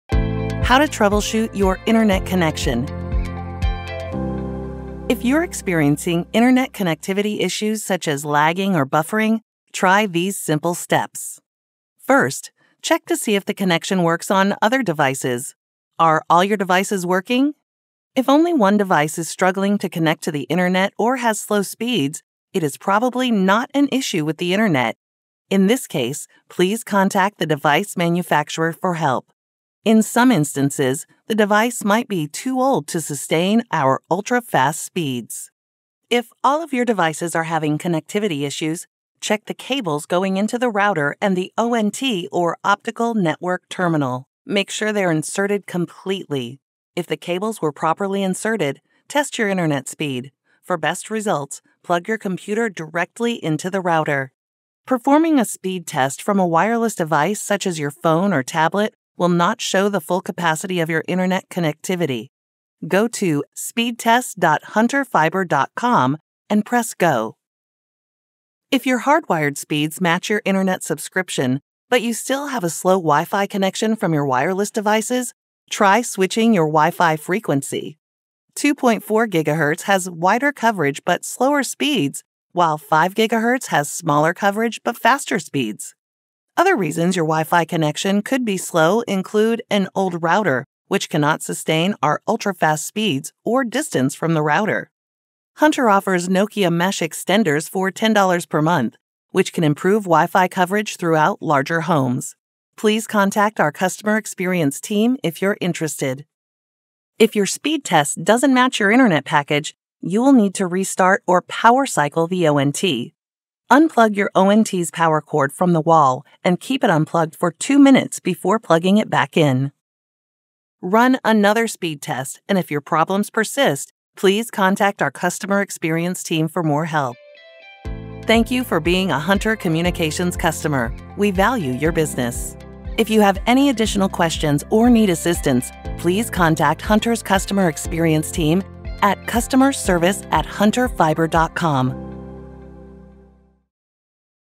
How to troubleshoot your internet connection
This video helps Hunter's fiber-optic internet customers troubleshoot their internet connection.